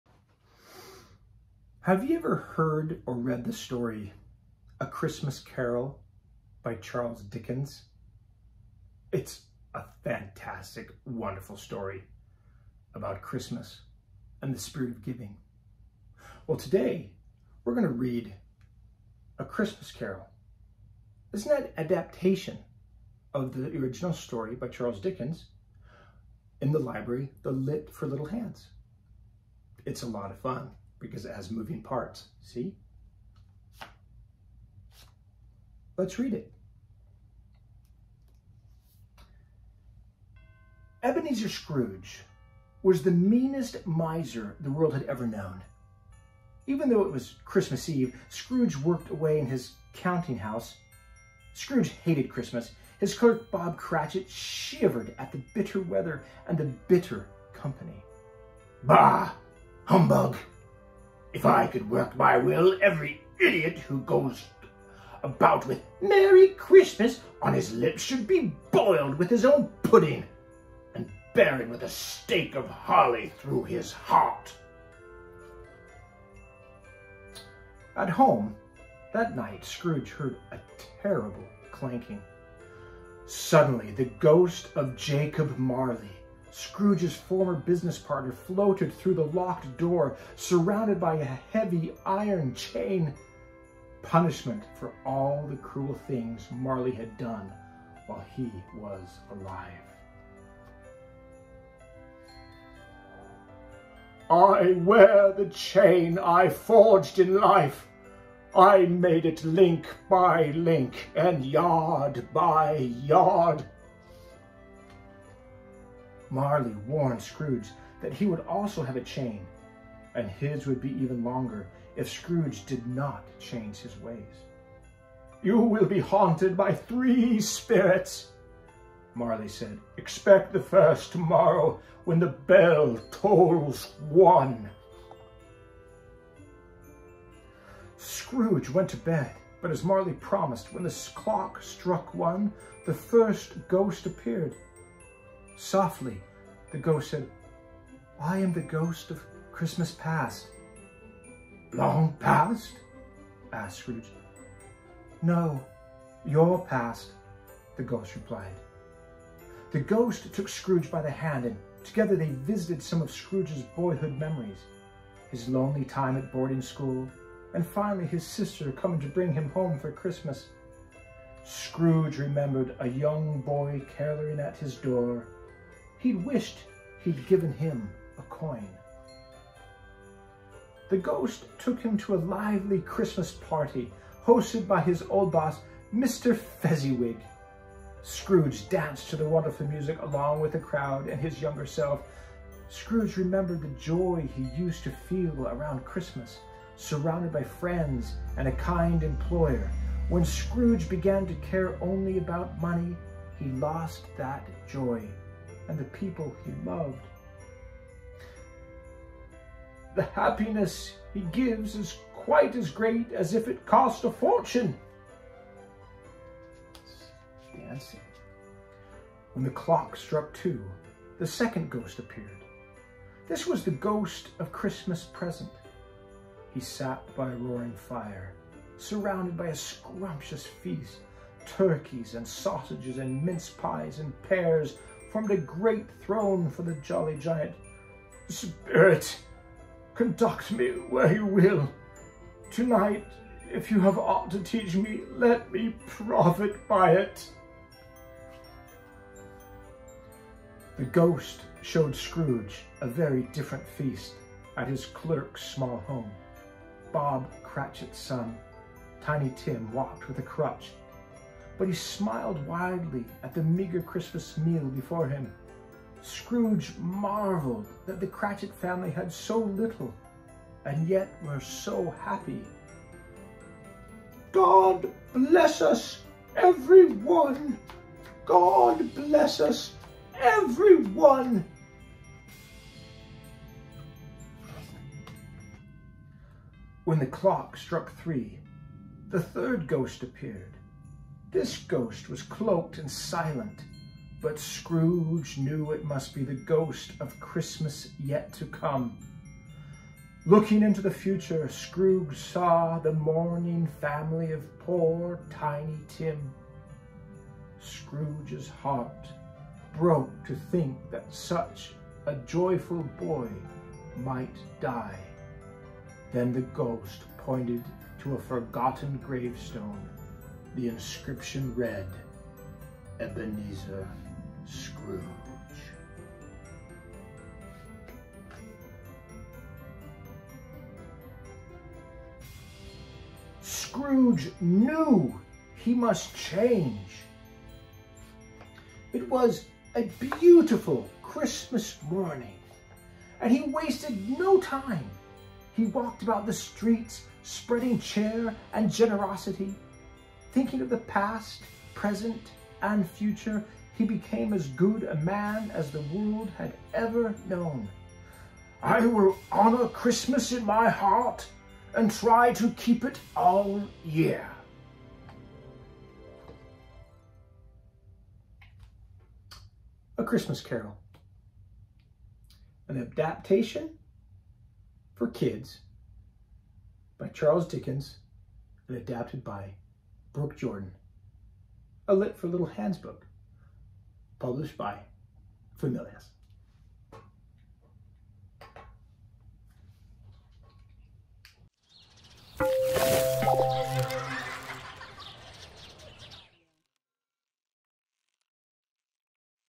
A Christmas Carol: Lit for Little Hands Adaptation | Read Along With Me Picture Books
Bah! Humbug! Charles Dickens’ classic Christmas ghost story, a tale of human nature and second chances, comes to life for children! Tons of interactive elements invite kids to follow the miserly Ebenezer Scrooge as he is haunted by the Ghosts of Christmas and learns the value of kindness. Much more than a primer, Lit for Little Hands: A Christmas Carol tells the actual story in simple, engaging prose, and fans of the novella will be delighted by the book’s attention to detail and clever use of original dialogue. And the book’s use of super-sturdy board means everyone can enjoy this tale of Christmas spirit over . . . and over . . . and over again!

Connect with out other YouTube account @Familius